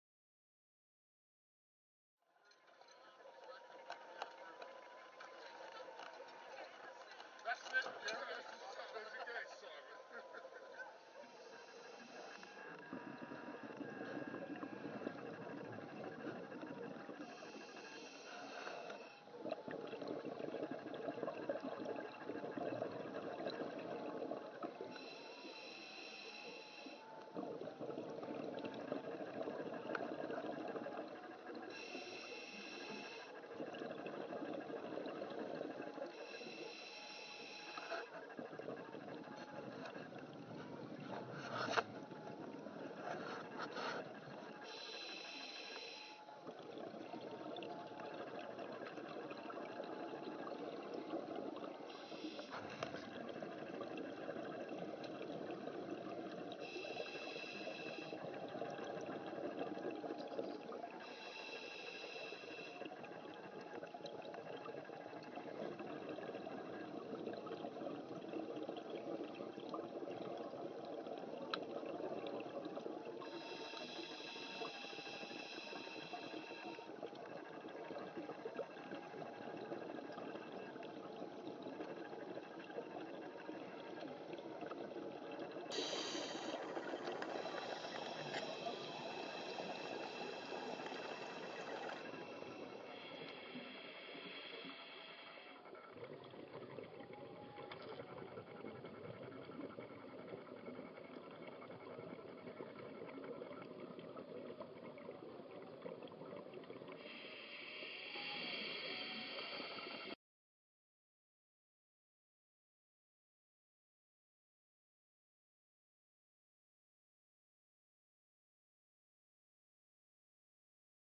Dive trip off Selsey
A short video clip from my first UK dive off East Beach at Selsey